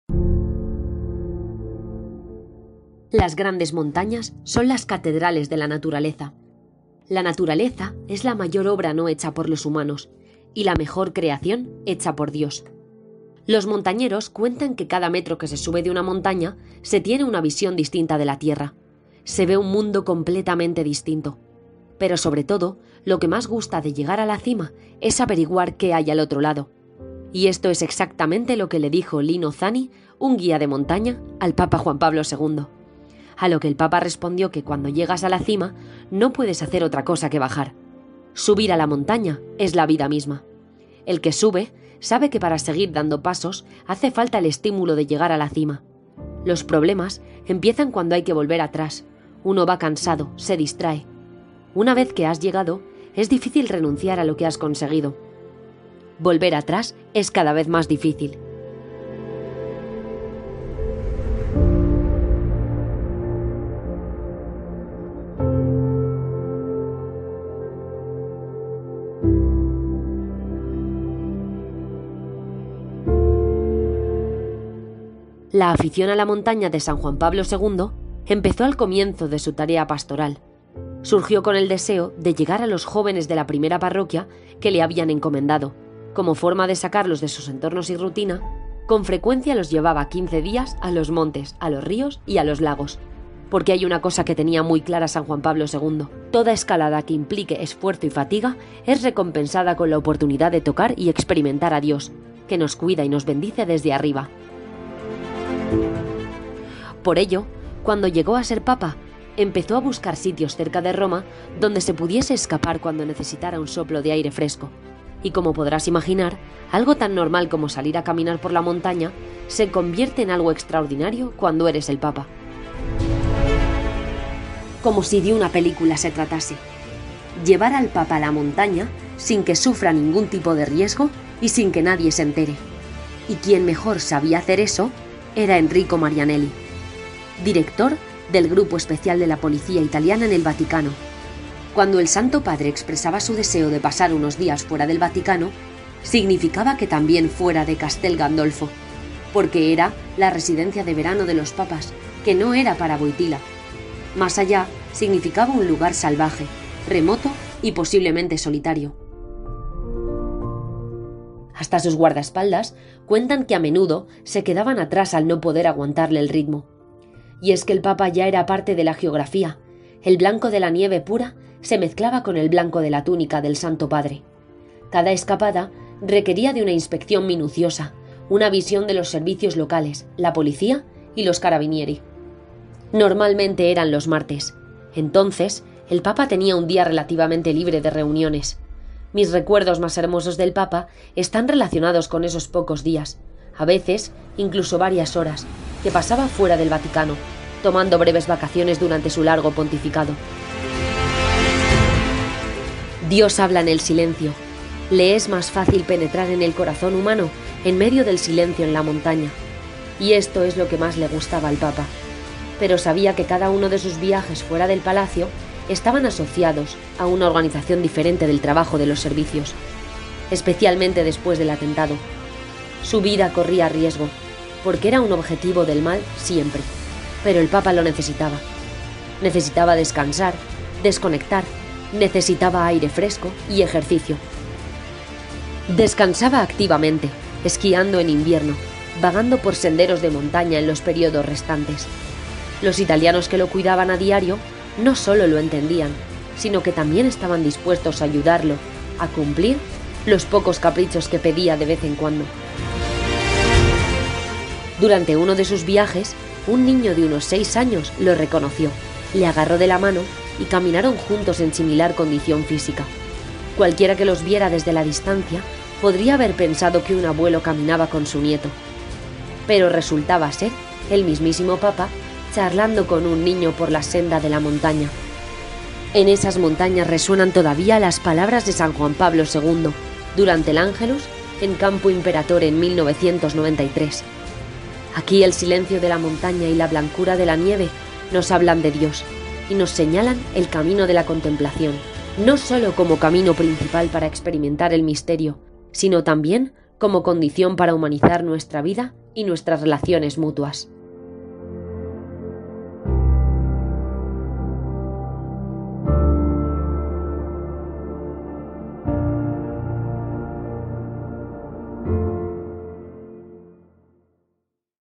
😍Las ESCAPADAS SECRETAS de SAN JUAN PABLO II
El Papa Juan Pablo II era un gran deportista y no desperdiciaba las oportunidades de escaparse del Vaticano para hacer su afición favorita: el montañismo.

Las grandes montañas son las catedrales de la naturaleza, la naturaleza es la mayor obra no hecha por los humanos y la mejor creación hecha por Dios. Los montañeros cuentan que cada metro que se sube de la montaña se tiene una visión diferente de la tierra.

Se ve un mundo completamente distintos pero sobre todo lo que más gusta de llegar a la cima es ver lo que hay al otro lado y esto es exactamente lo que le dijo Lino Zani, un guía de montaña al papa Juan Pablo II. A lo que el papa respondió que cuando llegas a la cima no puedes hacer otra cosa que bajar...

Y es que subir a la montaña es la vida misma, el que sube sabe que para seguir dando pasos hace falta el estímulo de llegar a la cima. Los problemas empiezan cuando hay que volver atrás: uno va cansado,se distrae. Una vez que has llegado, es difícil renunciar a lo que has conseguido. Volver atrás es cada vez más difícil.

San Juan Pablo II. Hacia la cima de la santidad

La afición a la montaña de San Juan Pablo II empezó al comienzo de su tarea pastoral. Surgió con el deseo de llegar a los jóvenes de la primera parroquia que le habían encomendado como forma de sacarlos de sus entornos y rutinas con frecuencia los llevaba quince días a los montes, a los ríos y a los lagos. Porque había una cosa que tenía muy clara San Juan  Pablo II: "Toda escalada que implique esfuerzo y fatiga es recompensada con la oportunidad de tocar y experimentar a Dios que nos cuida y nos bendice desde arriba".

Por ello, cuando llegó a ser papa empezó a buscar sitios cerca de Roma donde se pudiese "escapar" cuando necesitara aire fresco y como podrás imaginar, algo tan normal como salir a caminar por la montaña, se convierte en algo extraordinario cuando eres el papa.

No te pierdas el video completo.